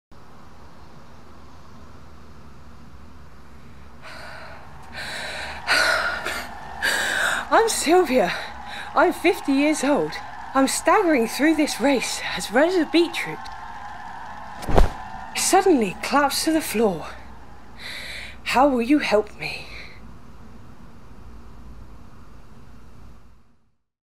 Paramedic Case Studies by Richard Pilbery
A stop motion animation of one of the case study scenarios within the book. How will you help Sylvia?